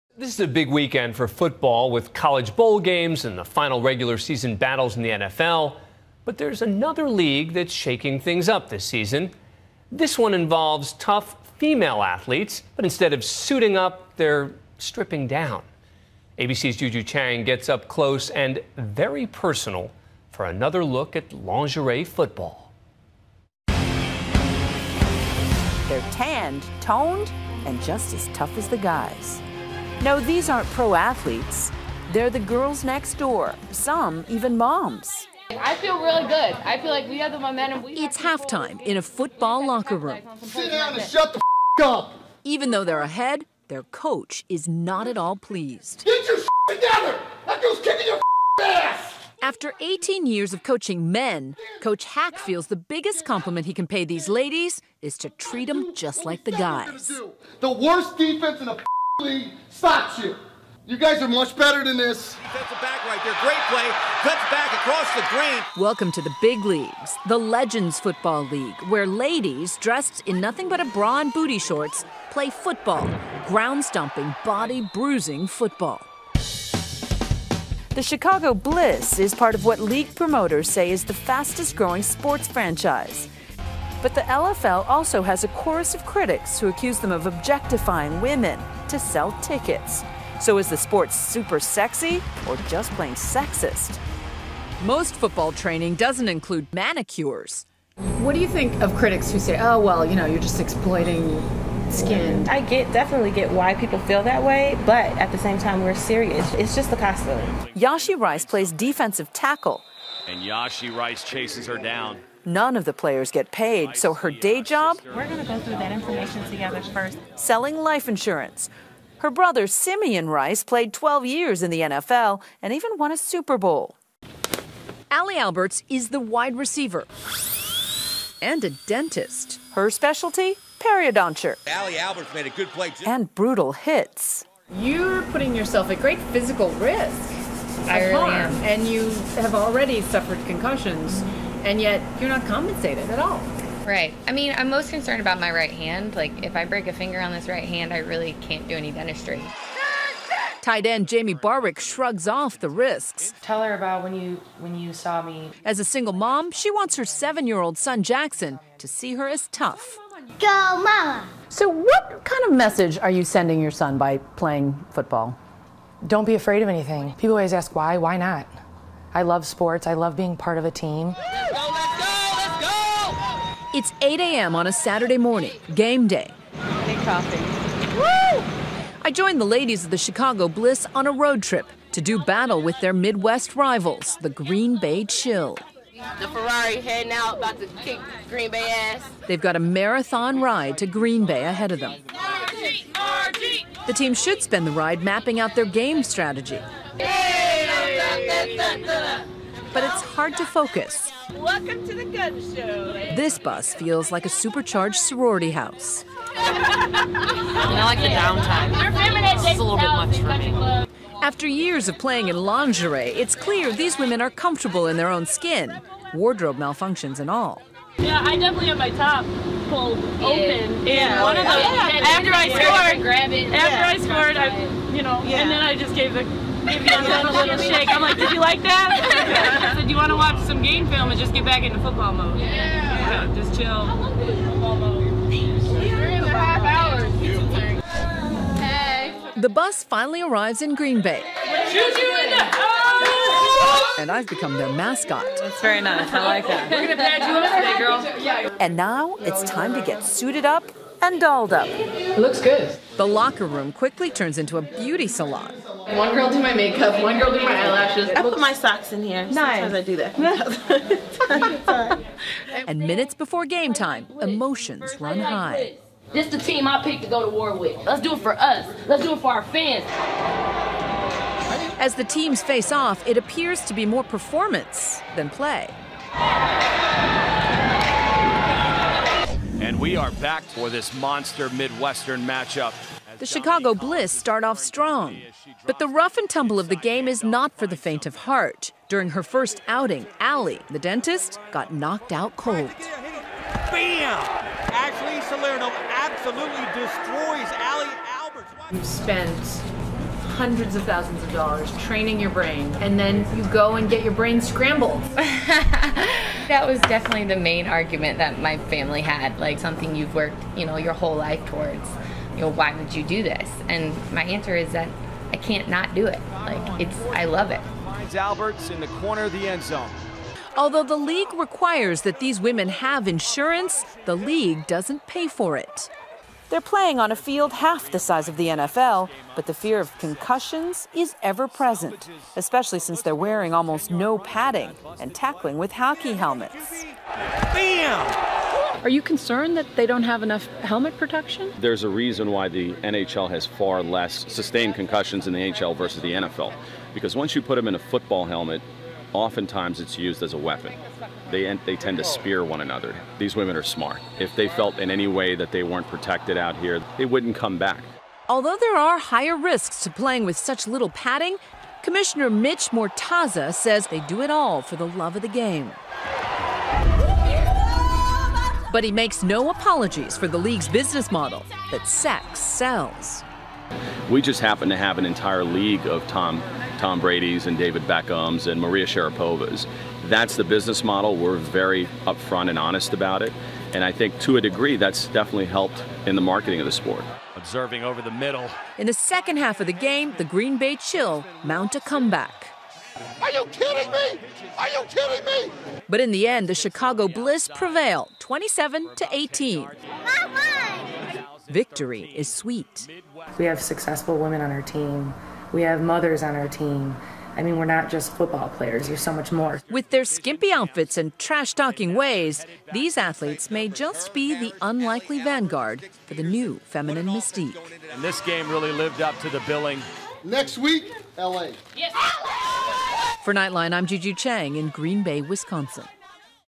Lingerie Football: Super Sexy or Sexist
Players in the all-female league wear skimpy uniforms and hockey helmets.